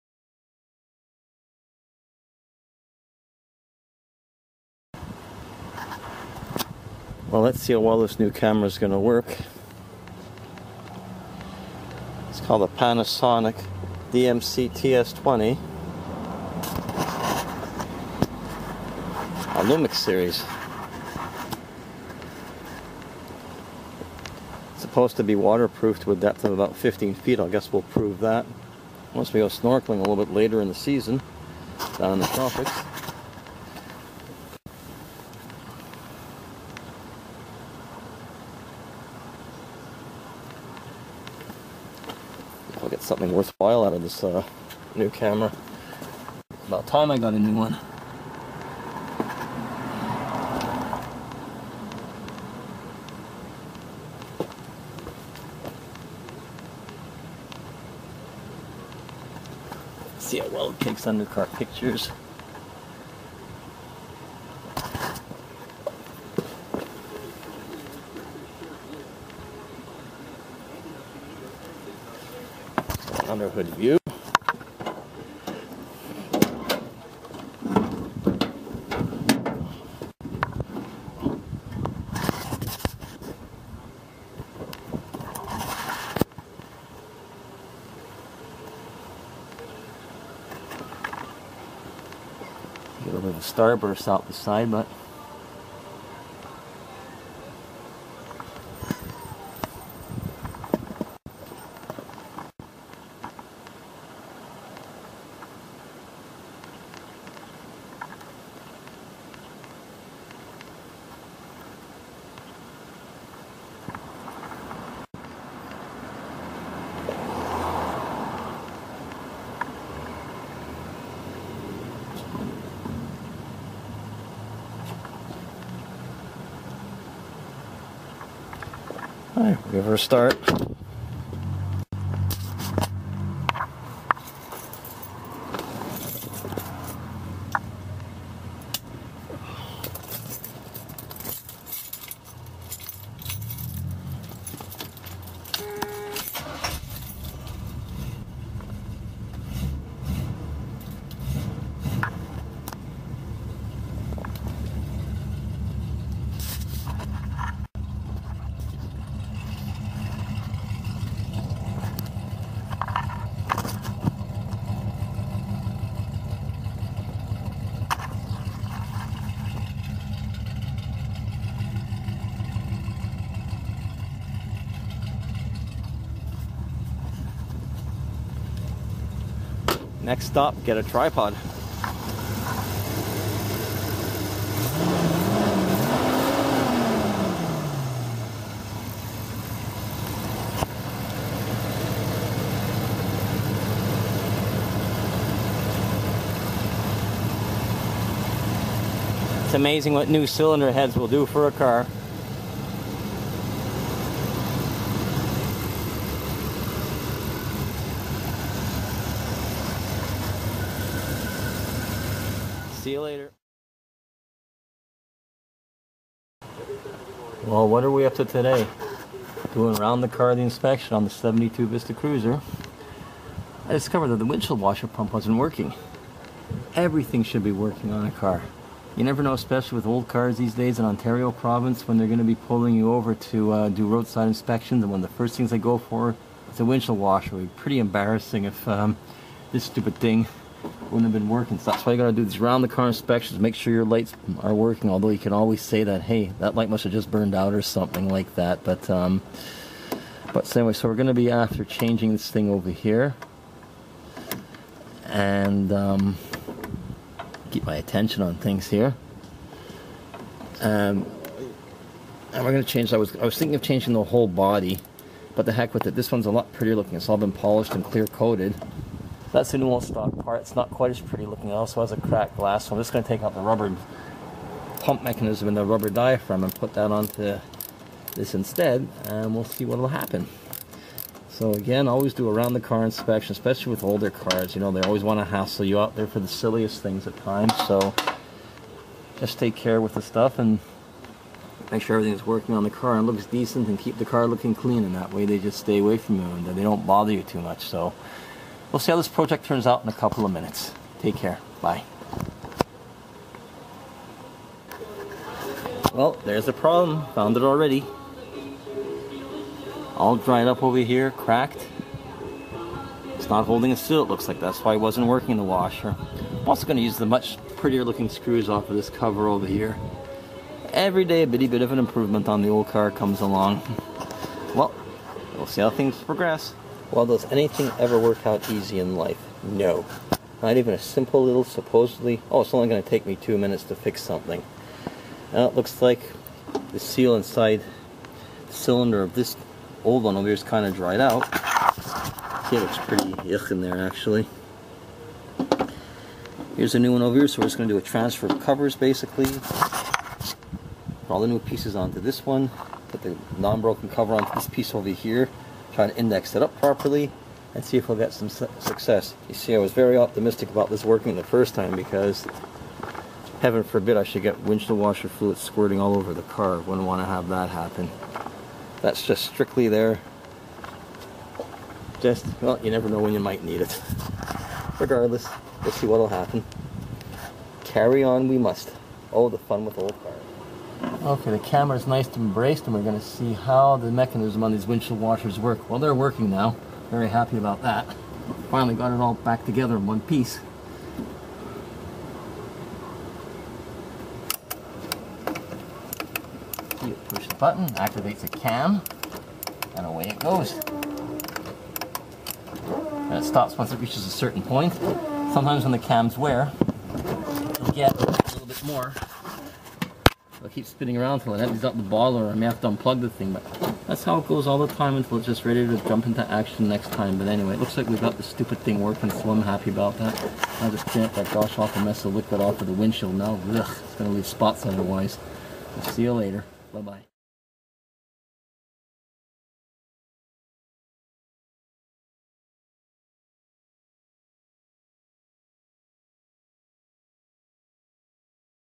Vista Cruiser, camera test and washer pump fix!
Just a small sundry of things, picked up a camera, will see how it goes with it, it will suit my lifestyle in the meantime, meaning I will be able to bring some under water clips in the near future, the colors and sights in coral reefs are not easily matched for their beauty. For the meantime, a simple few clips hastily pasted together to get to know the camera, and a needed fix for the washer pump on the 1972 Oldsmobile Vista Cruiser.